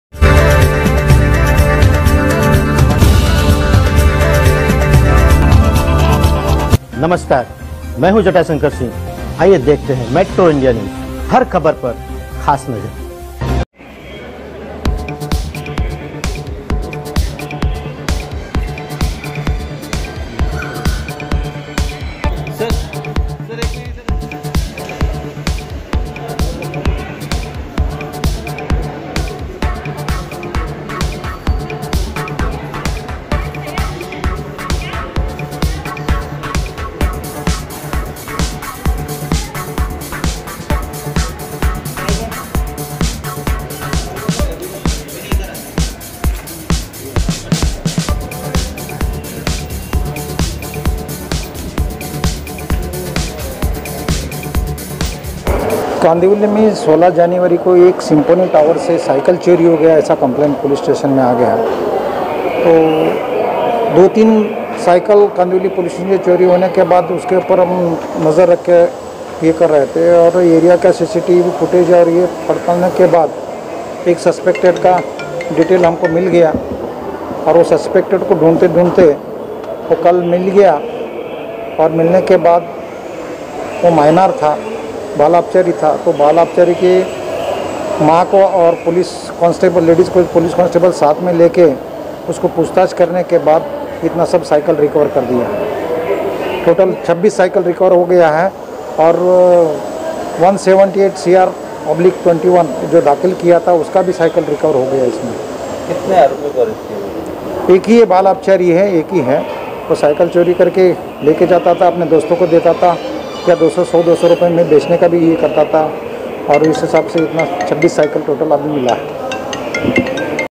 स्पोट्स साइकिल चोर का कारनामा || spots cyecil ki chori karanewala aresst
चोरी की 26 स्पोर्ट्स महेंगी सायकिलों के साथ आरोपी गिरफ्तार

मुम्बई उपनगर के हाई प्रोफाइल इलाके से महेंगी स्पोर्ट्स की साइकिलें चोरी कर एक हजार से 500 रुपये रुपये में बेचने वाले को 26 सायकिलों के साथ कांदिवली पुलिस की टीम ने गिरफ्तार किया है,

कांदिवली पुलिस से मिली जानकारी के मुताबिक पकड़ा गया आरोपी 16 साल का नाबालिग युवक है, और 9वीं की पढ़ाई करता है, छोटी से उम्र में लगी नशे की लत ने छात्र को चोर बना दिया है,

पकड़े गए आरोपी के पास से एक दो नही बल्कि चोरी की महेंगी और स्पोर्ट्स की 26 साइकिलें पुलिस ने बरामद किया है, जिसकी कीमत तीन लाख 90 हजार रुपये बताई जा रही है,

यह सभी सायकिलें कांदिवली, मालाड, बांगुर नगर, चारकोप के हाई प्रोफइल इलाकों से पिछले  दो महीने में चोरी की गयी थी।

कांदिवली पुलिस ने कुछ लोगों को उनकी चोरी की सायकिल उन्हें वापस भी लौटाया, जिसे पाकर वह काफी खुश नजर
 आए, और कांदिवली पुलिस का धन्यवाद किये।

फ़िलहाल कांदिवली पुलिस आरोपी को गिरफ्तार कर अब इस बात की जांच कर रही है कि इसके साथ चोरी में हाथ साथ देने वाले और कितने लोग शामिल है, और कितनी और सायकिलों की चोरियां की है।